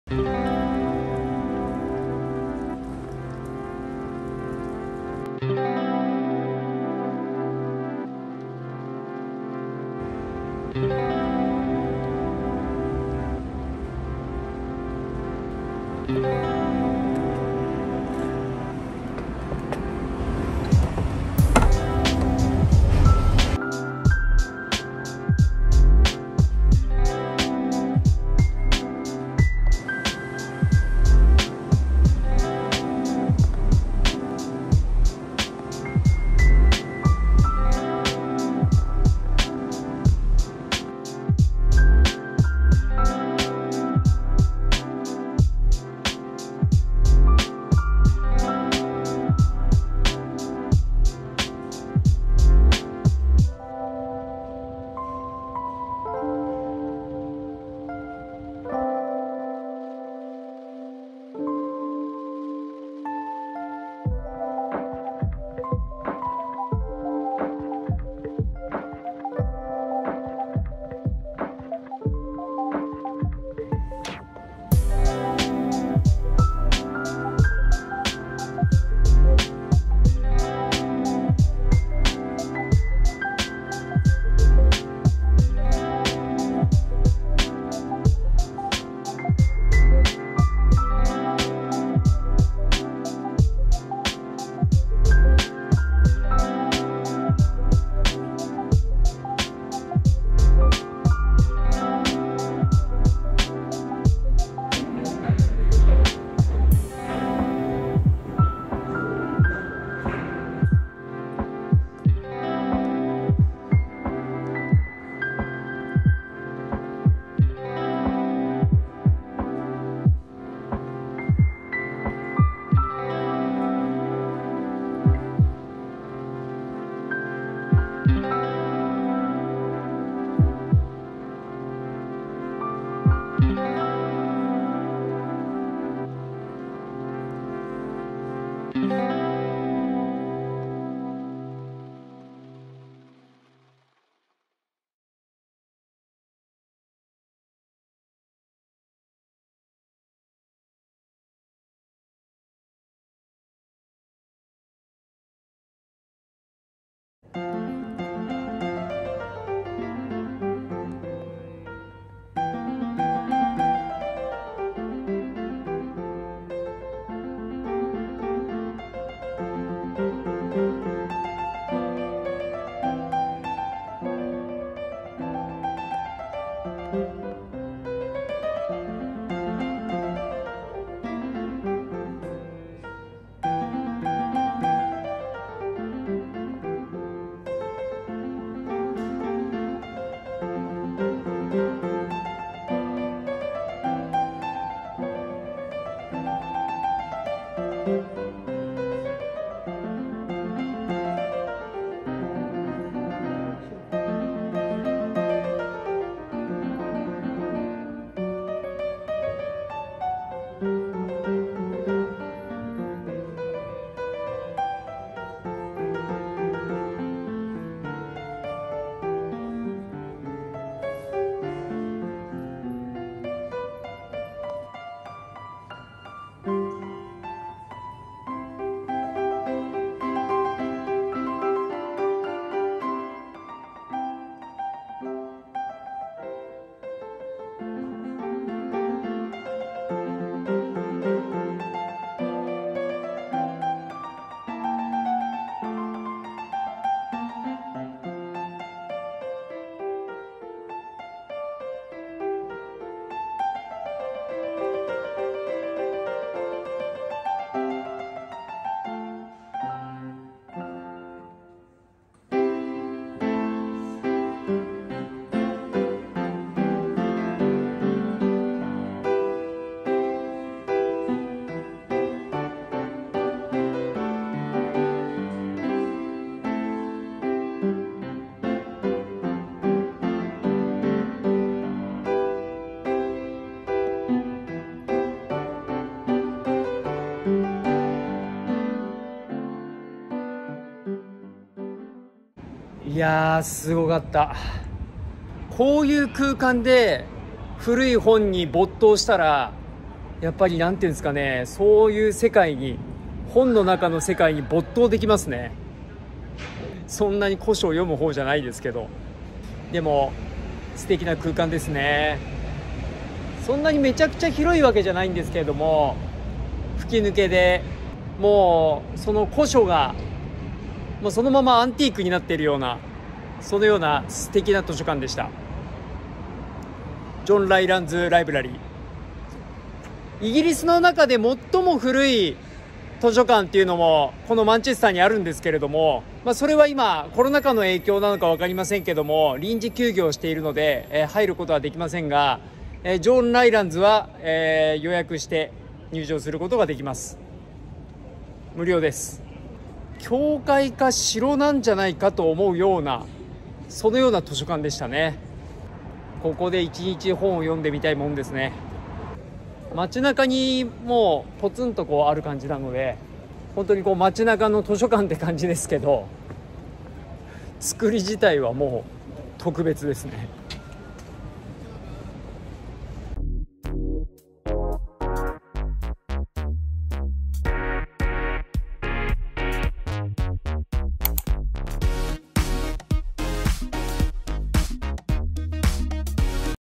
【UK】John Rylands Library 〜世界一美しい図書館〜
マンチェスターの街の図書館…というには荘厳すぎる
ジョンライランズライブラリは世界一美しい図書館の一つ。
創設者夫婦が敬虔なクリスチャンで多くの寄付や社会貢献をしてきたがそのひとつがこの図書館。
建物だけでなく創設者の思いも美しい。